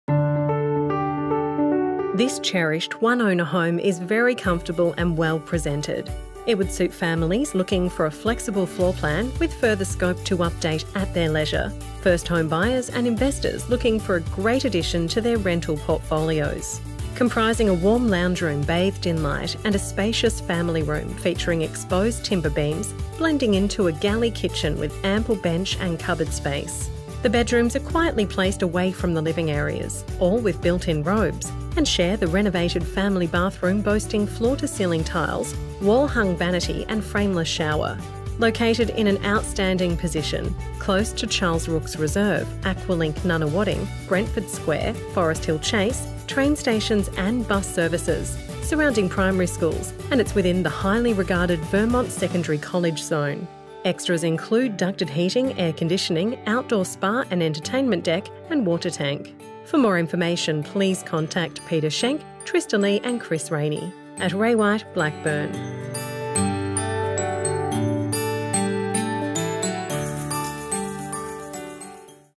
63 Betula Avenue, Vermont
This cherished one owner home is now being offered for sale for the first time in 56 years. Very comfortable and well presented, it would suit families looking for a flexible floorplan with further scope to update at their leisure, first home buyers and investors looking for a great addition to their rental portfolios.

The interior comprises warm lounge room bathed in light, a spacious family room featuring exposed timber beams, blending into a galley kitchen with ample bench and cupboard space. The three bedrooms are quietly placed away from the living areas, all with built in robes and share the renovated family bathroom boasting floor to ceiling tiles, contemporary wall hung vanity and frameless shower.

Highlights: ducted heating, air conditioning, outdoor spa and entertainment deck, single lock up garage with workshop and water tank. Located in an outstanding position, it’s close to Charles Rooks Reserve, Aqualink Nunawading sporting complex, Brentford Square, Forest Hill Chase, Mitcham and Nunawading train stations, buses from Canterbury Road, St Timothy’s Catholic Primary School, Mt Pleasant Road and Vermont Primary Schools, and it’s within the highly regarded Vermont Secondary College zone. Well cared for, this is an opportunity not to be missed.